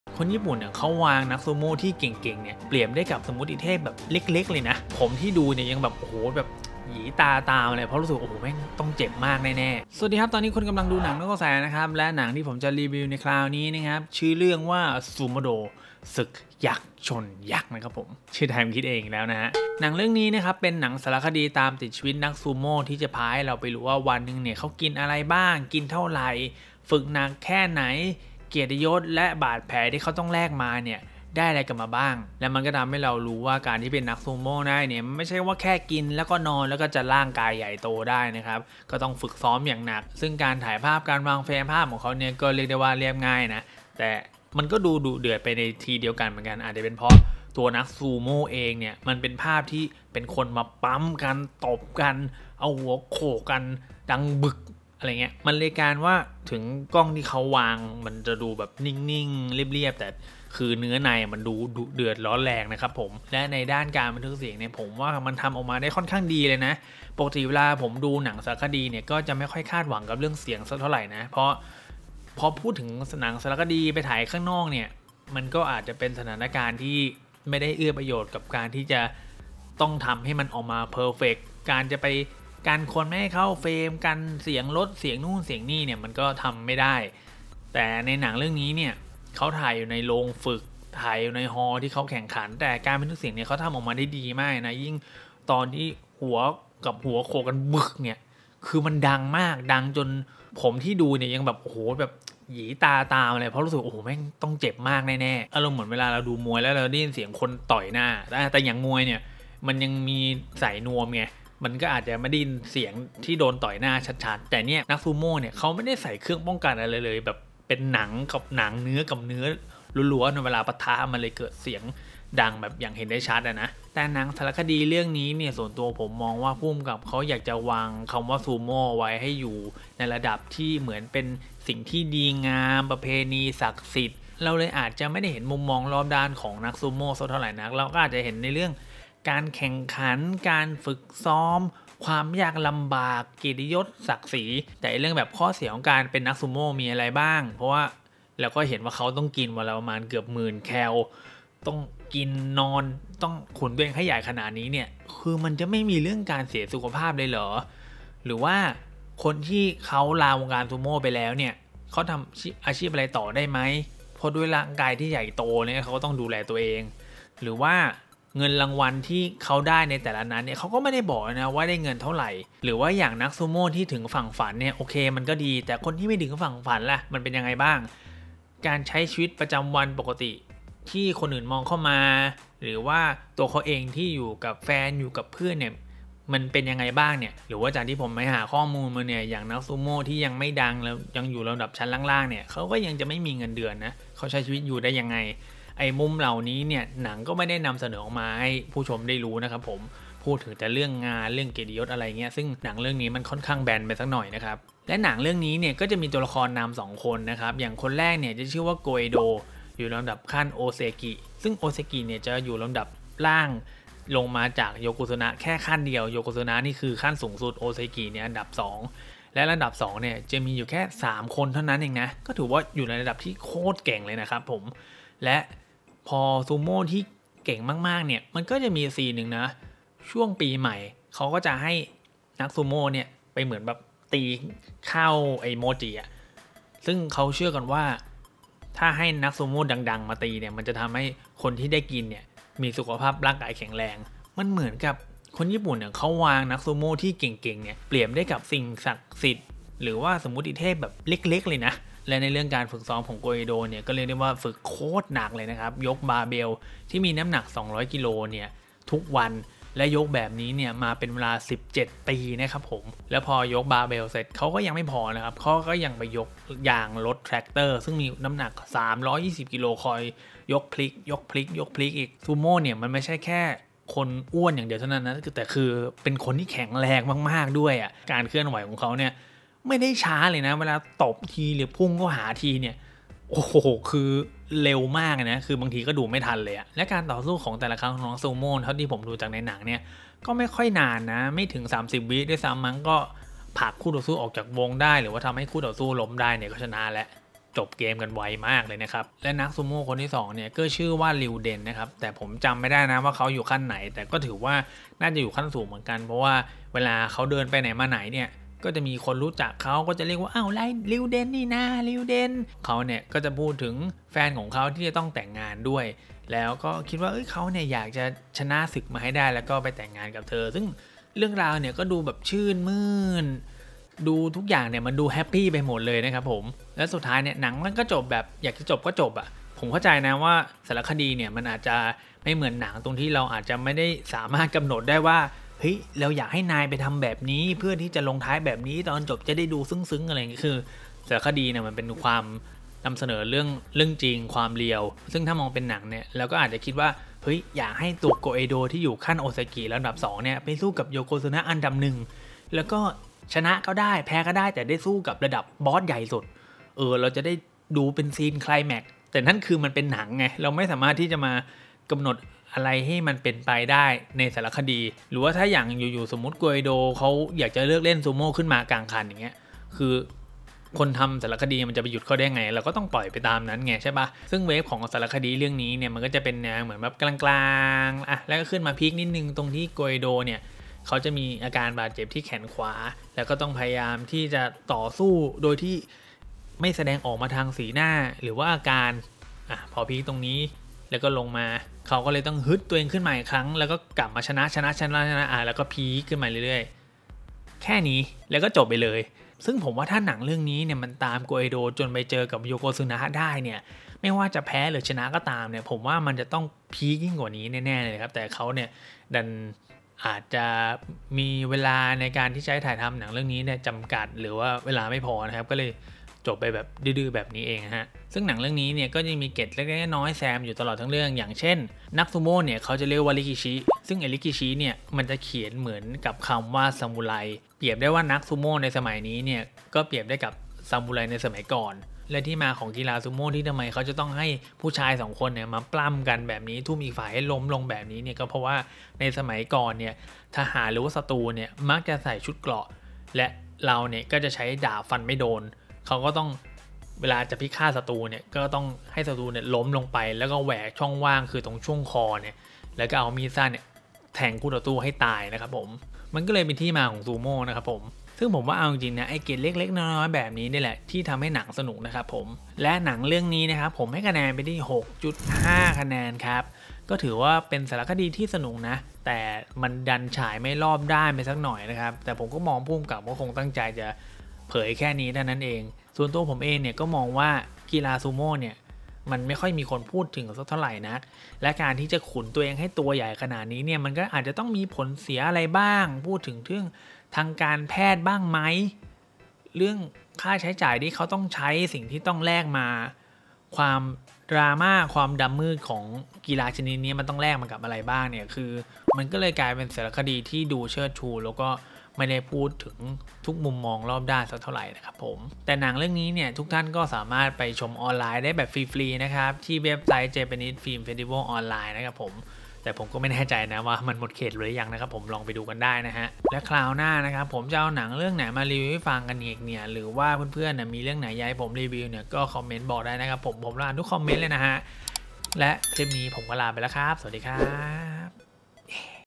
รีวิวหนัง Sumodo ศึกยักษ์ชนยักษ์ ฝึกโหดมาก! / ดูหนังนอกกระแส
เมื่อการเป็นซูโม่ไม่ได้แค่กินและนอนแต่ต้องฝึกหนักทุกวัน และต้องแลกมากับการบาดเจ็บแสนสาหัส!

พูดคุย ติดต่อลงโฆษณา เป็นกำลังใจกันได้ที่

ติดตามอัพเดตเราได้ที่นี่